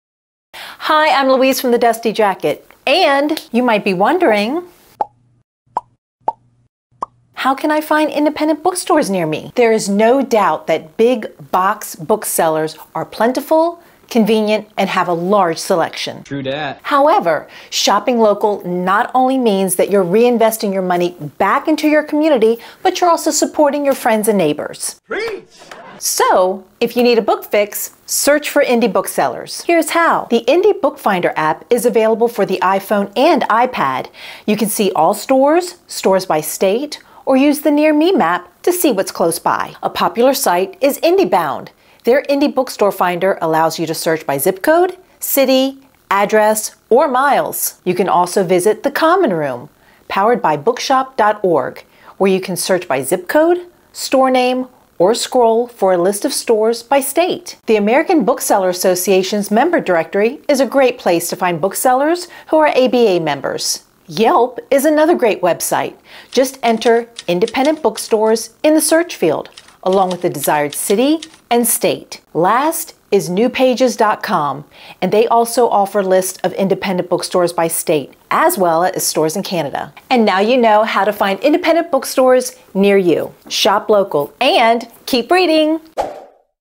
How Can I Find Indie Bookstores Near Me?
Finding an independent bookseller close to you is easier than ever with these handy resources!

Sound effects credit: Pixabay.com